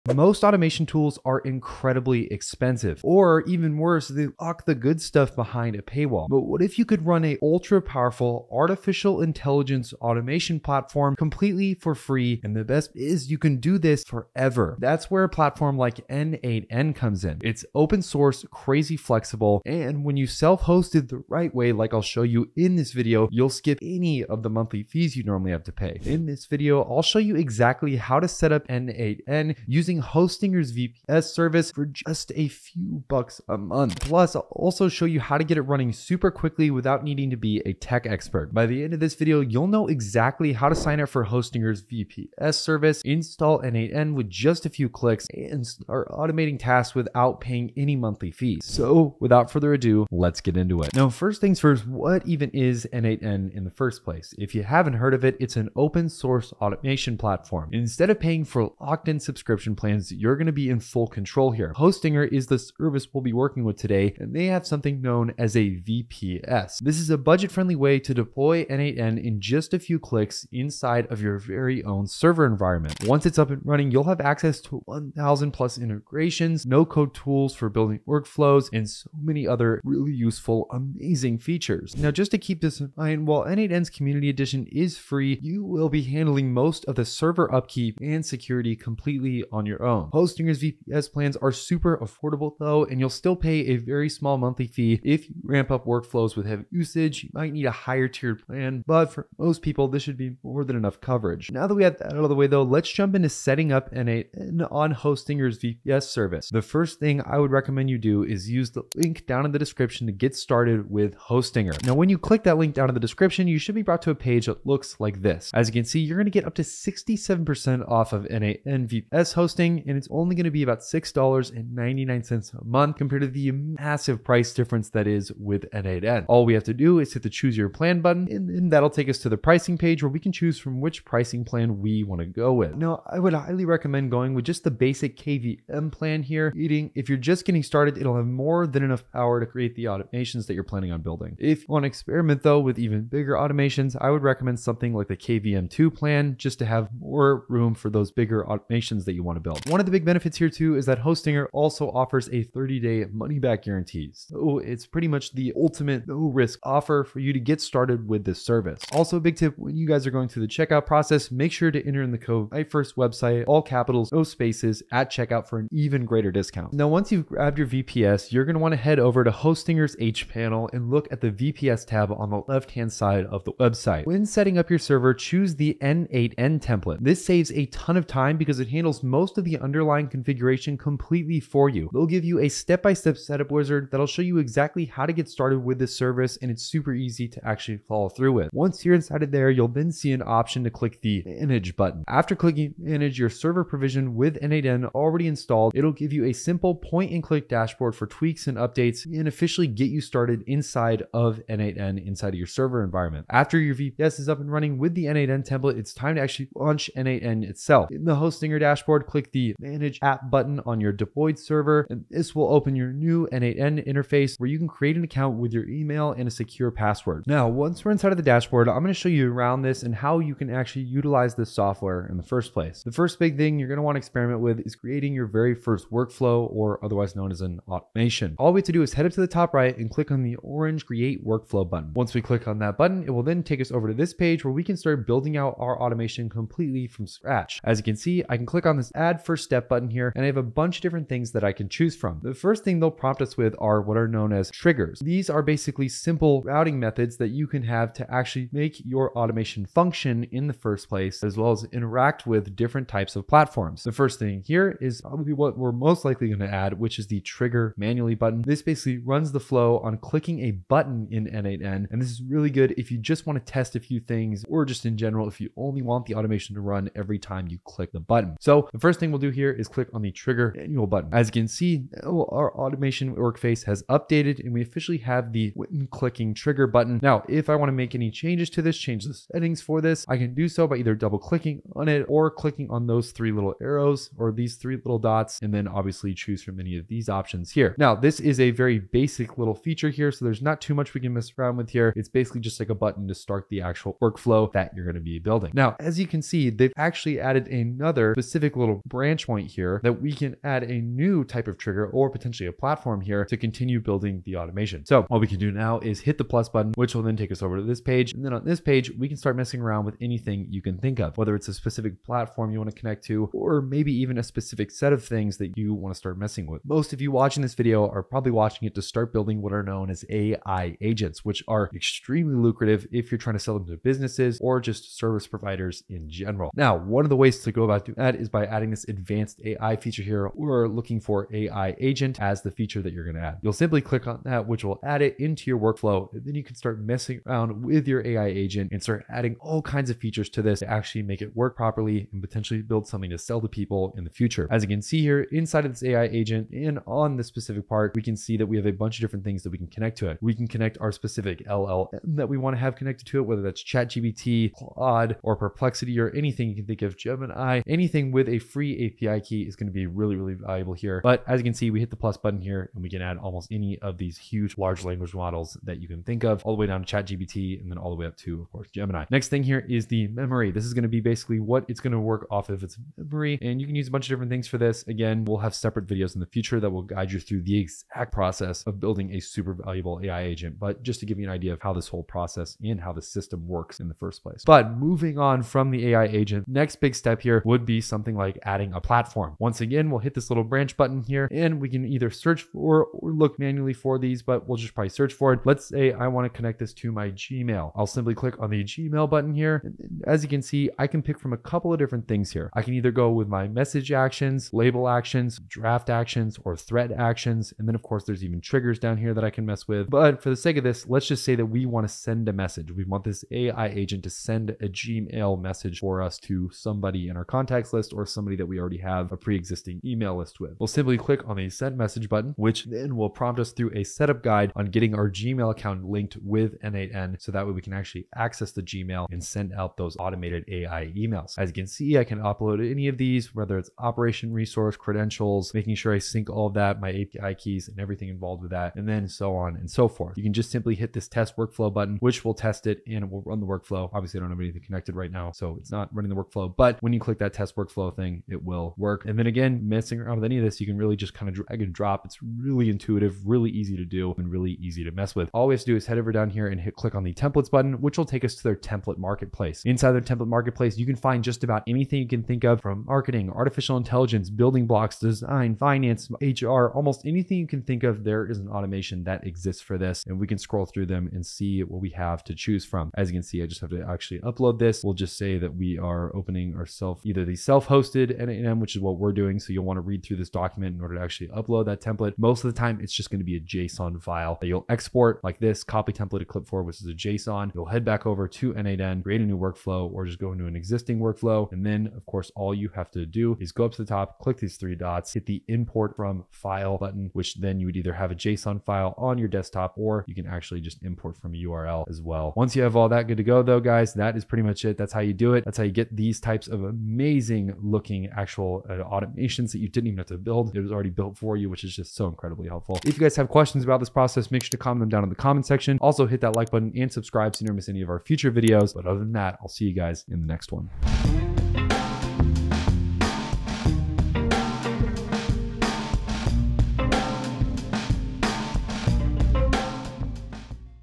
How to Use n8n for FREE with Hostinger VPS (No Monthly Fees)
Automate anything with n8n, with 400+ app integrations, no code needed, totally free:

In this video, I’ll go over how to use n8n’s open-source automation platform for free by setting it up on Hostinger’s VPS. You’ll learn how to install n8n in just a few clicks, skip monthly subscription fees, and run AI-powered workflows from your own server. This is a great way to build powerful automations using tools like OpenAI, Google Sheets, Gmail, Notion, and more.

Be sure to watch this video all the way as I cover step-by-step how to sign up for VPS hosting, launch the n8n template, and get your dashboard up and running. I’ll walk you through how to use pre-built workflow templates, connect popular apps, and automate tasks without writing code. You’ll also learn how to fix common setup mistakes and keep your server safe and secure. This tutorial makes it easy to start automating tasks without paying for Zapier, Make, or other subscription-based tools.

n8n is a free, open-source automation tool with over 1,000 integrations and no hidden fees. You can self-host it using VPS services like Hostinger to unlock full control and save money each month. With n8n, you get low-code features, AI-powered actions, and full flexibility for custom workflows. It’s a smart way to build automations for startups, freelancers, side hustles, or small teams looking to cut software costs.

AI Academy is here to help you learn new things, keep up with AI, and use it in everyday life.

– Kyle

Timeline:
0:00 - Introduction
0:51 - What is n8n?
1:02 - Why Use Hostinger VPS for n8n?
1:22 - Things to Consider in n8n
1:52 - Getting Started with Hostinger
2:39 - Setting Up Your Hostinger Account
3:37 - Walkthrough of Using n8n
7:35 - Using Pre-built Templates
8:32 - Conclusion